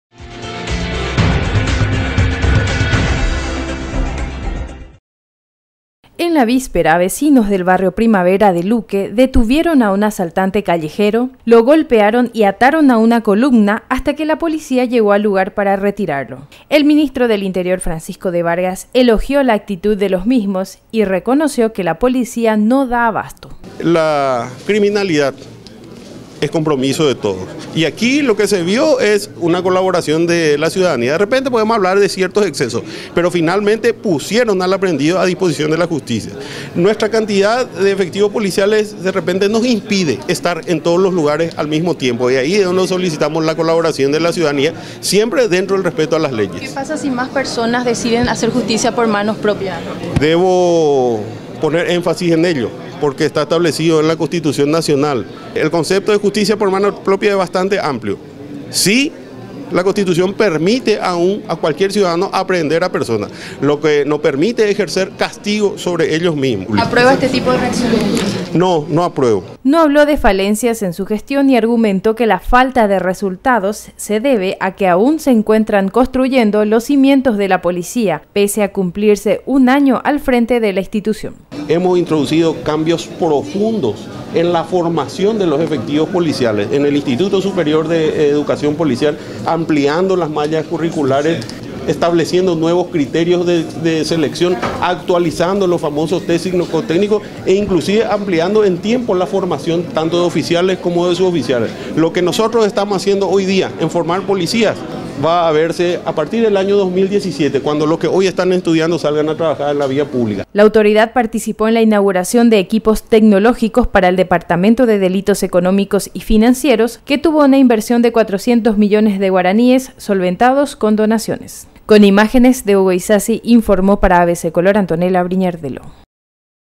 De Vargas reconoce que policías no dan abasto
Vecinos del barrio Primavera de Luque hicieron justicia por manos propias al detener a golpes a un asaltante. Esta reacción fue elogiada por el ministro del Interior, Francisco de Vargas, quien reconoció que los policías no dan abasto.